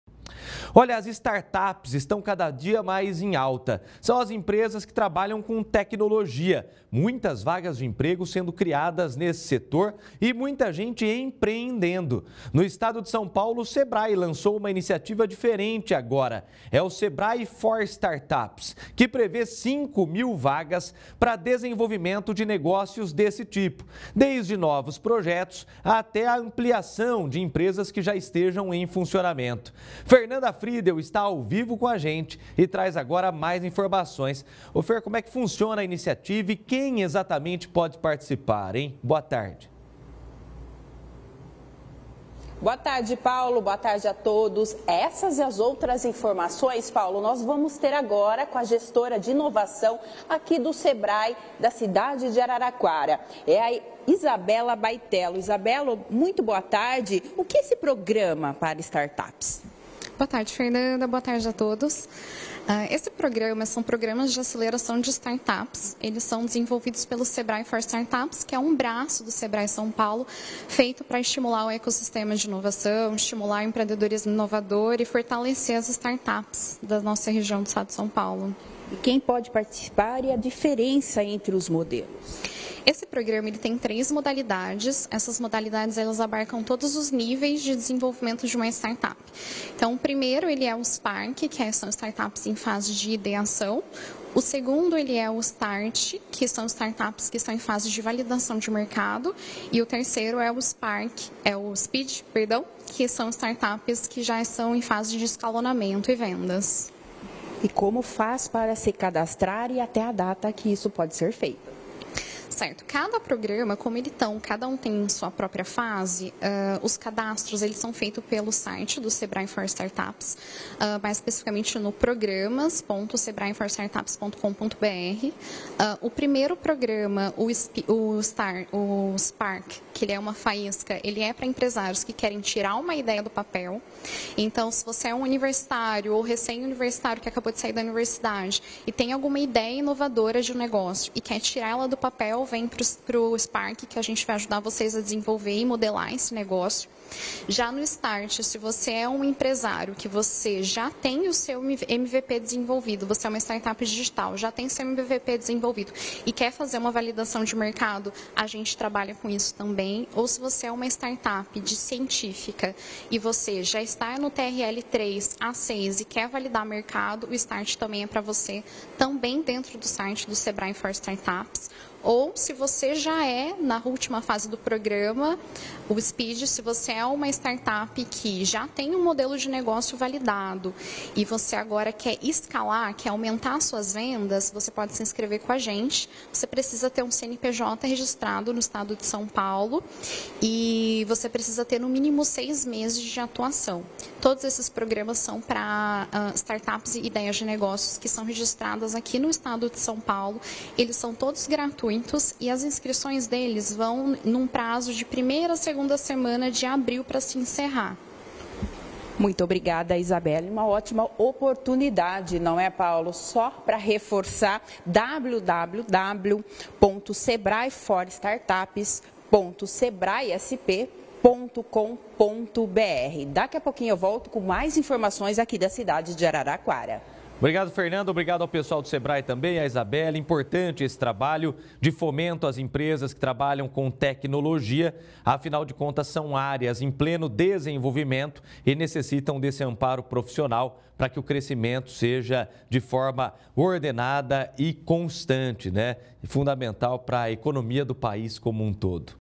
CRESCIMENTO DAS STARTUPS (26/03/2024)
As startups estão cada vez mais em alta. No estado de São Paulo, o SEBRAE lançou a iniciativa 'SEBRAE For Startups', que prevê 5 mil vagas para desenvolvimento de negócios, desde o projeto até aquelas startups que já estão mais avançadas em funcionamento.